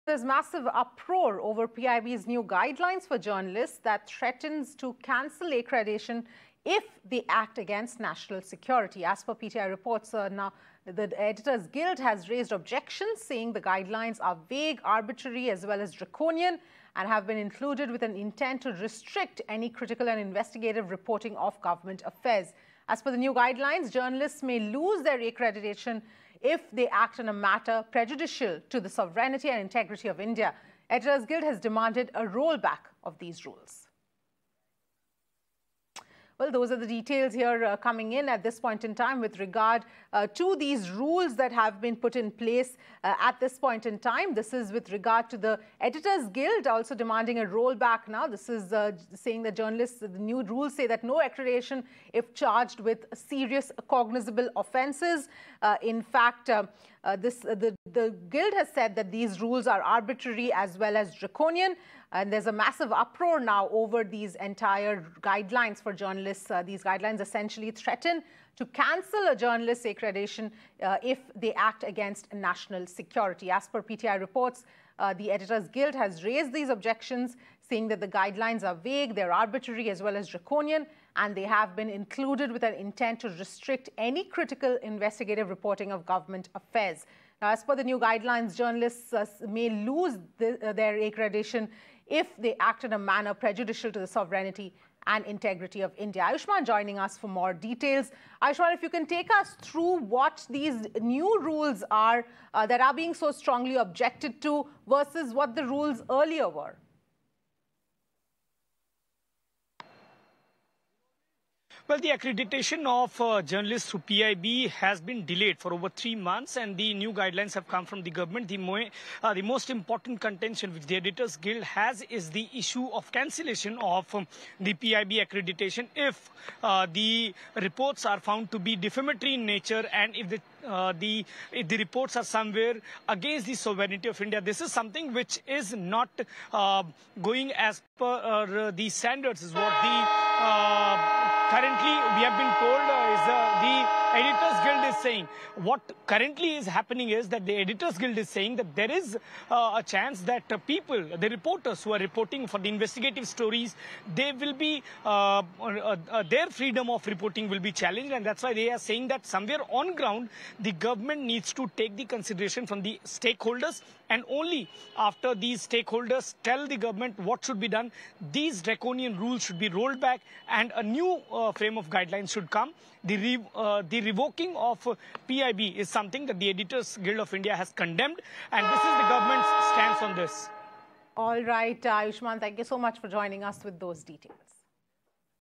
Editors Guild Demands Rollback Of New Journalist Accreditation Norms | Latest News Updates
In a massive uproar over PIB's new guidelines for journalists that threatens to cancel the accreditation of journalists and media organization if found to work against the national sovereignty and integrity, the Editors Guild has now raised strong objections. Stating the new norms as vague, arbitrary, and draconian, the Editors Guild has demanded the revoking of the new norms. The Guild has also claimed that the new guidelines are put in place to prohibit all sorts of critical and investigative journalism. The new guidelines that have been introduced say that anyone working against the national interest will lose their government accreditation.